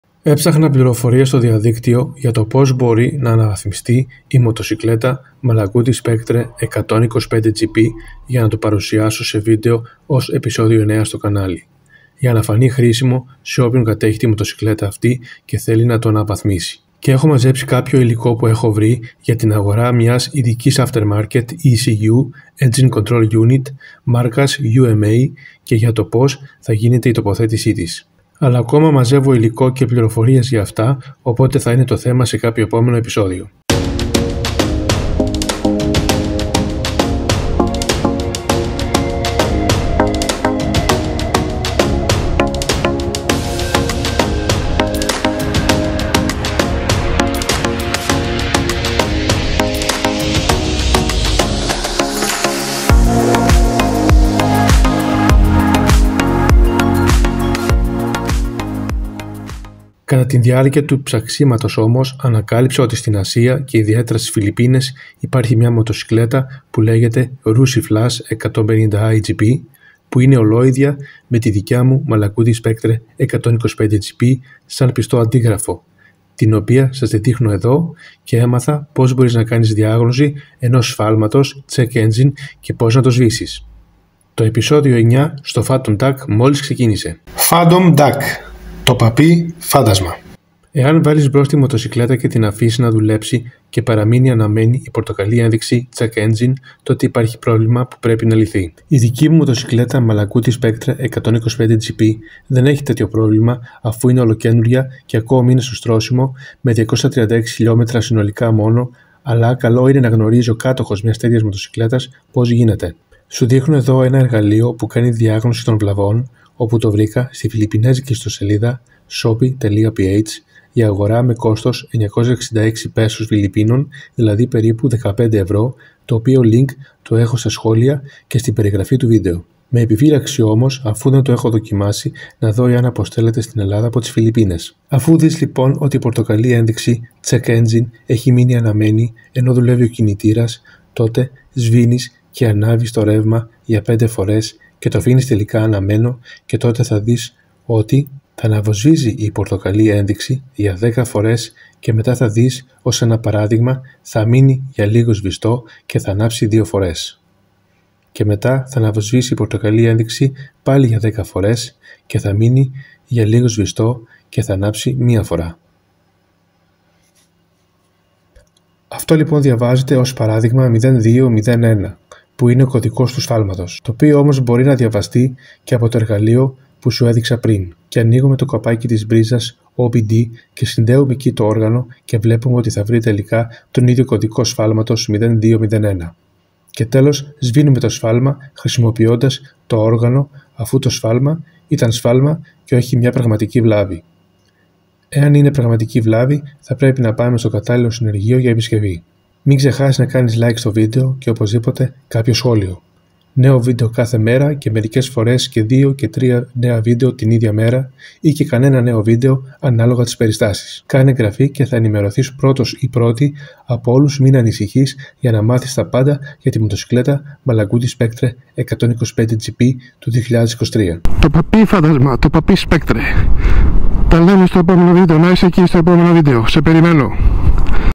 Πως να κανεις | S1E09 | διαγνωση & σβησιμο σε Check Engine σε Malaguti Spectre 125 GP | Phantom Duck
Πως να κανεις | S1E09 | διαγνωση & σβησιμο σε Check Engine σε Malaguti Spectre 125 GP | Phantom Duck.  Εψαχνα πληροφοριες στο διαδυκτιο για το πως μπορει να αναβαθμιστει η μοτοσυκλετα Μalaguti Spectre 125 GP, για να το παρουσιασω σε βιντεο ως επεισοδιο 9 στο καναλι, για να φανει χρησιμο σε οποιον κατεχει την μοτοσυκλετα αυτη,
και θελει να το αναβαθμισει.
Και εχω μαζεψει καποιο υλικο που εχω βρει, για την αγορα μιας
ειδικης aftermarket ECU, Engine Control Unit, μαρκας UMA, και για το πως γινεται η τοποθετηση της,  αλλα ακομα μαζευω υλικο και πληροφοριες για αυτα, οποτε θα ειναι το θεμα σε καποιο επομενο επεισοδιο.
Κατα την διαρκεια του ψαξιματος ομως, ανακαλυψα οτι στην Ασια,
και ιδιαιτερα στις Φιλιππινες, υπαρχει μια μοτοσυκλετα
που λεγεται Rusi Flash 150i GP, που ειναι ολοιδια με την δικια
μου Malaguti Spectre 125 GP, σαν πιστο αντιγραφο, την οποια σας την δειχνω εδω, και εμαθα πως μπορεις να κανεις διαγνωση ενος σφαλματος Check Engine και πως να το σβησεις. Το επεισοδιο Εp.9 στο Phantom Duck, μολις ξεκινησε !
Εαν βαλεις μπρος την μοτοσυκλετα και την αφησεις να δουλευει και παραμεινει αναμμενη η πορτοκαλι ενδειξη check engine τοτε υπαρχει προβλημα που πρεπει να λυθει.
Η δικη μου Μοτοσυκλετα Μalaguti Spectre 125 GP, δεν εχει τετοιο
προβλημα αφου ειναι ολοκαινουργια και ακομη ειναι στο στρωσιμο με 000236 km συνολικα μονο, αλλα καλο ειναι να γνωριζει ο κατοχος μιας τετοιας μοτοσυκλετας πως γινεται.
Σου δειχνω εδω ενα εργαλειο που κανει διαγνωση των βλαβων,
οπου το βρηκα στην φιλλιπινεζικη ιστοσελιδα Shopee.ph για αγορα με κοστος 966 pesos φιλλιπινων, δηλαδη περιπου 15 ευρω, το οποιο λινκ το εχω στα σχολια και στην επιγραφη του βιντεο.
Με επιφυλαξη ομως, αφου δεν το εχω δοκιμασει να δω
εαν αποστελλεται στην ελλαδα απο τις φιλλιπινες.
Αφου δεις λοιπον οτι η πορτοκαλι ενδειξη ckeck engine εχει μεινει αναμενη , ενω δουλευει ο κινητηρας, τοτε σβηνεις
και αναβεις το ρευμα για 5 φορες και το αφηνεις τελικα αναμμενο, και τοτε θα δεις οτι θα αναβοσβησει η πορτοκαλι ενδειξη για 10 φορες και μετα θα δεις ως ενα παραδειγμα, θα μεινει για λιγο σβηστει και θα αναψει 2 φορες. και μετα θα αναβοσβησει η πορτοκαλι ενδειξη για 10 φορες παλι και θα μεινει για λιγο σβηστει και θα αναψει 1 φορα.
αυτο λειπον διαβαζεται ως παραδειγμα 0201 οπου ειναι ο κωδικος του σφαλματος,  το οποιο ομως μπορει να διαβαστει και απο το εργαλειο που σου εδειξα πριν.
Ανοιγουμε το καπακι της πριζας ΟΒD και συνδεουμε εκει το οργανο και βλεπουμεοτι θα βρει τελικα τον ιδιο κωδικο σφαλματος 0201.
Και τελος σβηνουμε το σφαλμα, χρησιμοποιωντας
το οργανο, αφου το σφαλμα, ηταν σφαλμα μονο και οχι πραγματικη βλαβη. Εαν ειναι πραγματικη βλαβη. θα πρεπει να παμε στο καταλληλο συνεργειο για επισκευη.

Νεο βιντεο καθε μερα, και μερικες φορες και δυο και τρια νεα βιντεο την ιδια μερα η και κανενα νεο βιντεο, αναλογα τις περιστασεις.